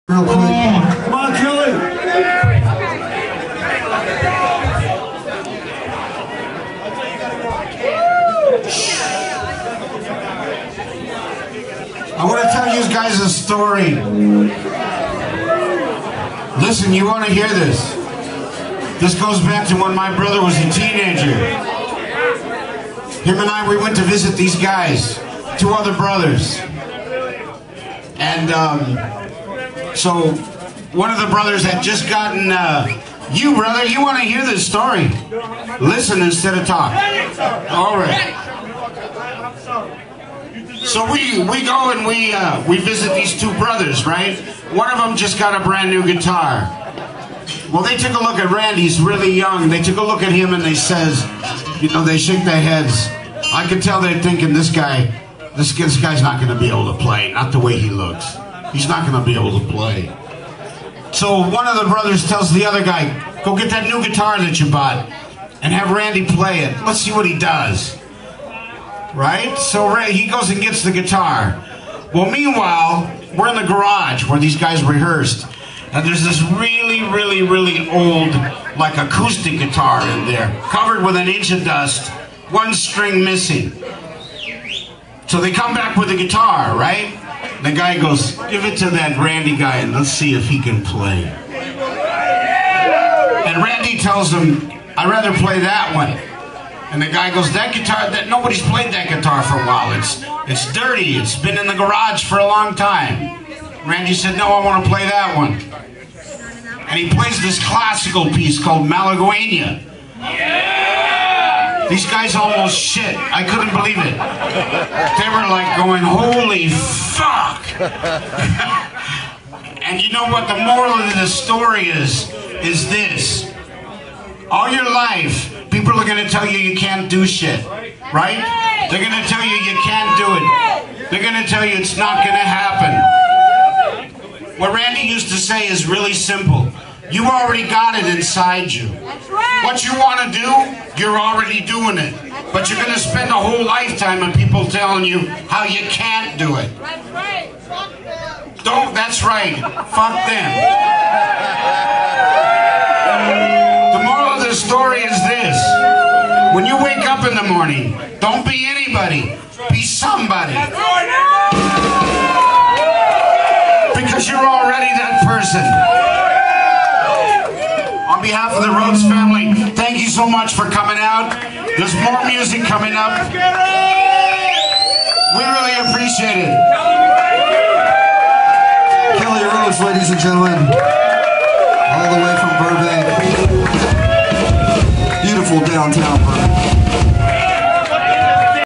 Kelle Rhoads
Kelle talks about Randy playing a dirty guitar...